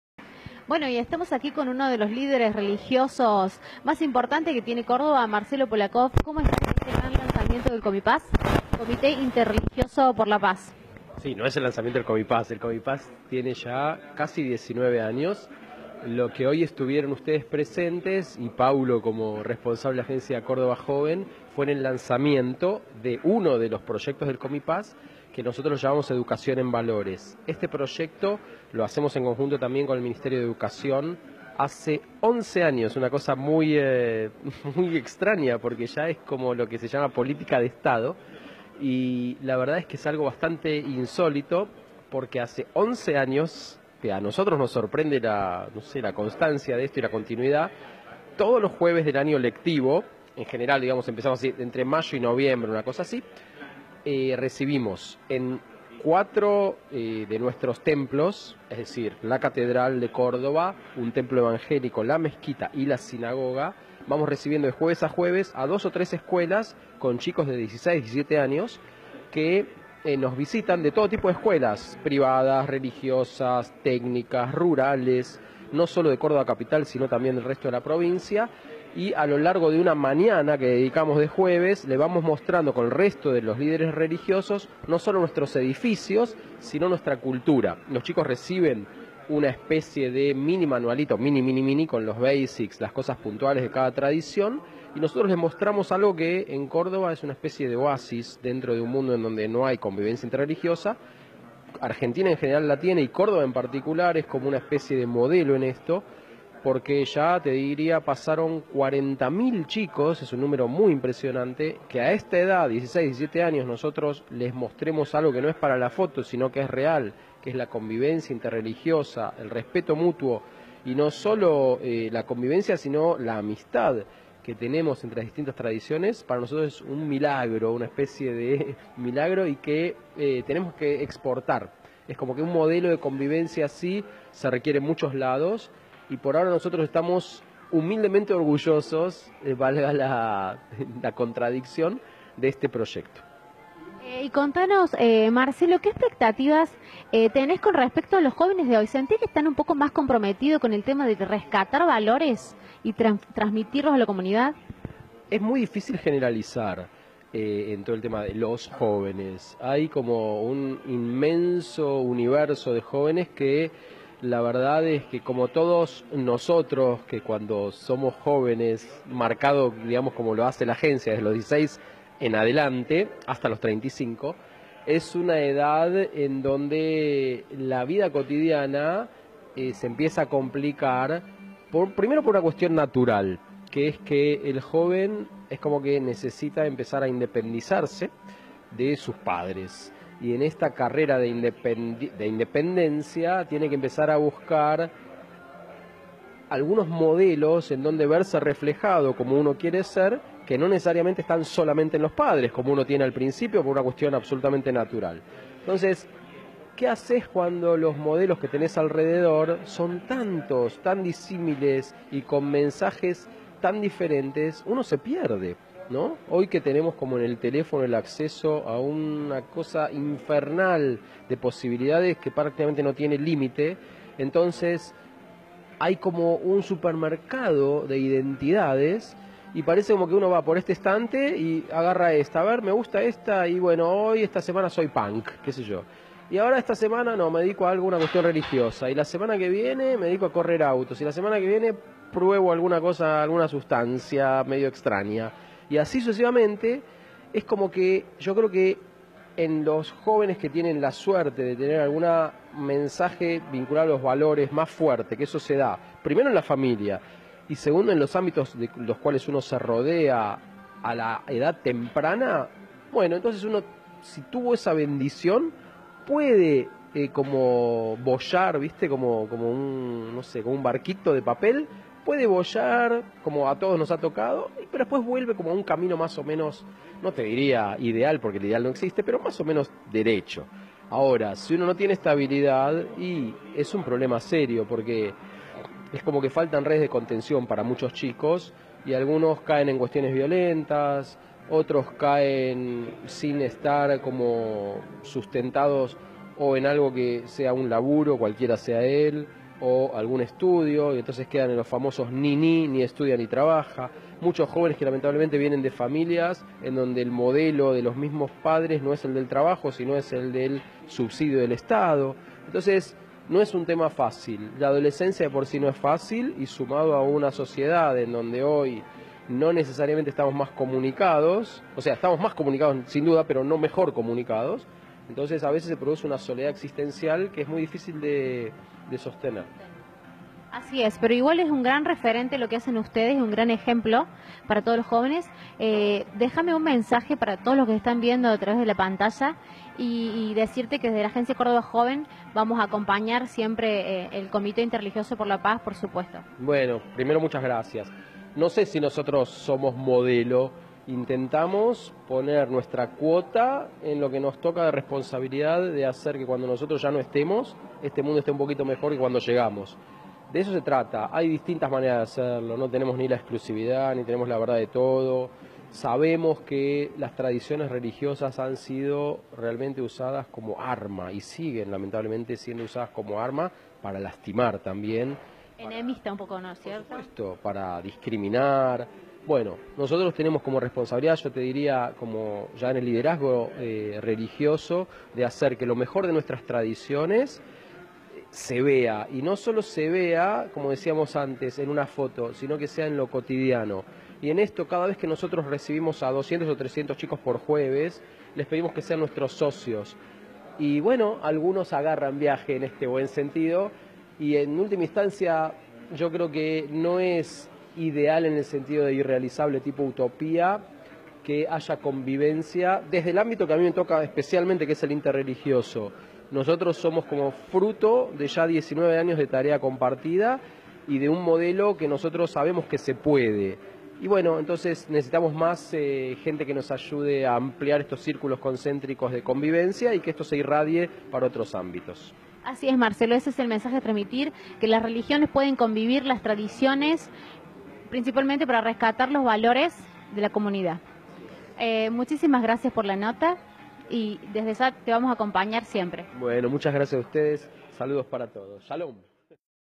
Lanzamiento de Educación en Valores
¡¡Gracias Rabino Marcelo Polakoff de COMIPAZ por contarnos todo sobre el lanzamiento del proyecto EDUCACION EN VALORES; nota exclusiva para La Docta Radio de la Agencia Cordoba Joven!!

La Docta! 🎧 Agencia Córdoba Joven